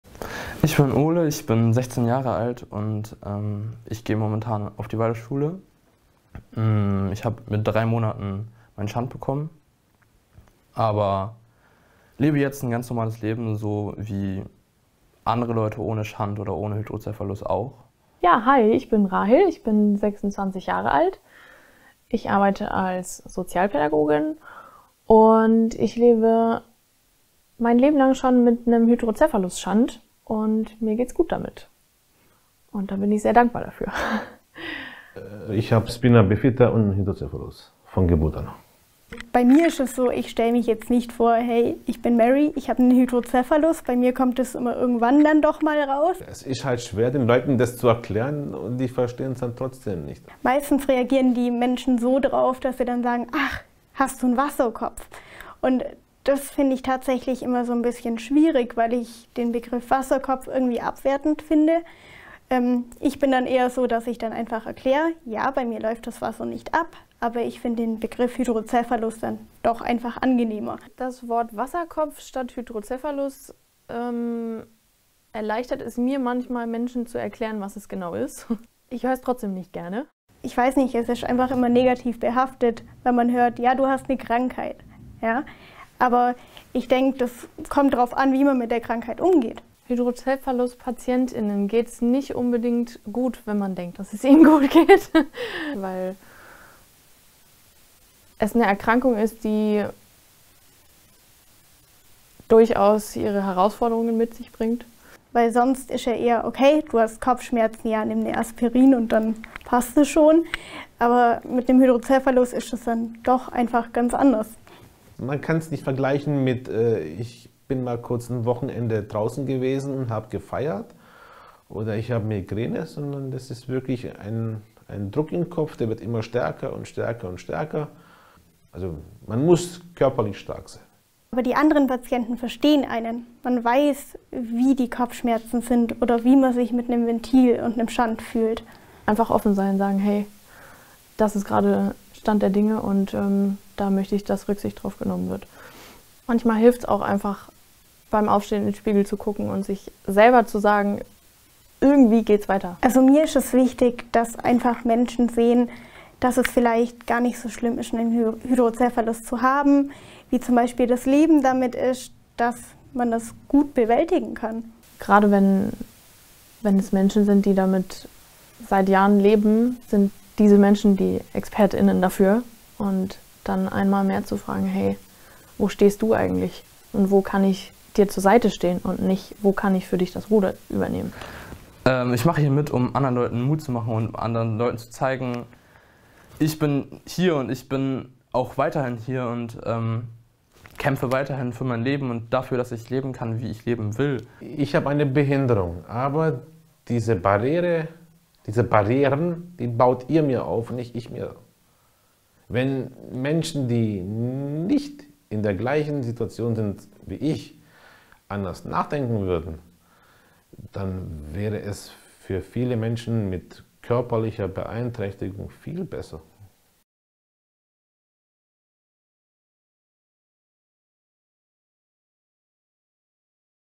Lasst uns Hydrocephalus sichtbarer machen
Vier Hydrocephalus Patient*innen mit verschiedenen Hintergründen erheben ihre Stimme: Wie wirkt sich ihr Hydrocephalus auf ihr Leben aus? Was hilft ihnen in schweren Zeiten? Was würden sie uns allen gerne mal über ihr Leben mit Hydrocephalus sagen? Zusammen können wir diese Krankheit, die kleine Babies genauso wie ältere Menschen treffen kann, bekannter machen!

Jeder Hydrocephalus ist individuell. Mit der MyHCandME Kampagne wollen wir die Geschichten und Gesichter hinter der Diagnose sichtbar machen. Mach mit und erzähle deine Geschichte!